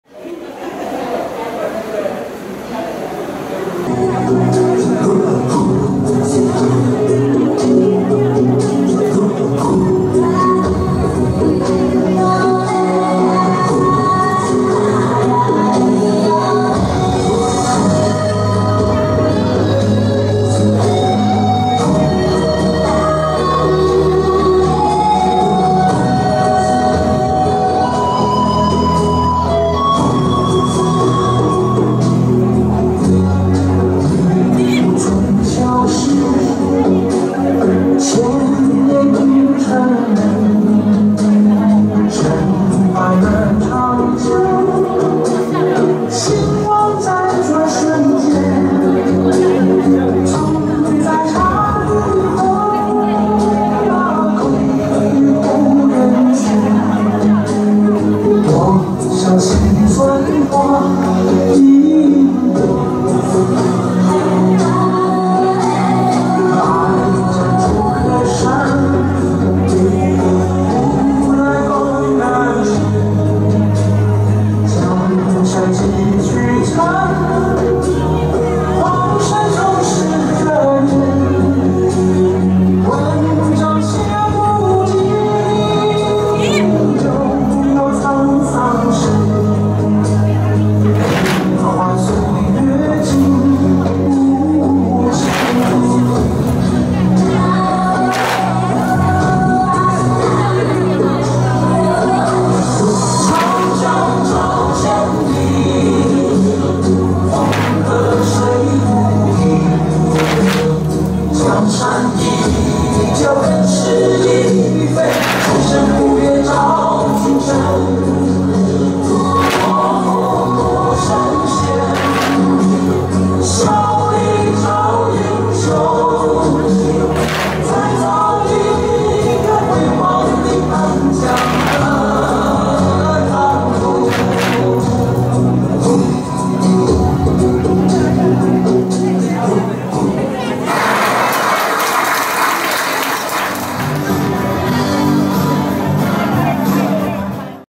112年樟文里文山太極拳武苑~~五步拳+五步扇
112年5月6日 文山區樟文里慶祝母親節活動
112年樟文里文山太極拳武苑~~五步拳+五步扇
#112.5.6日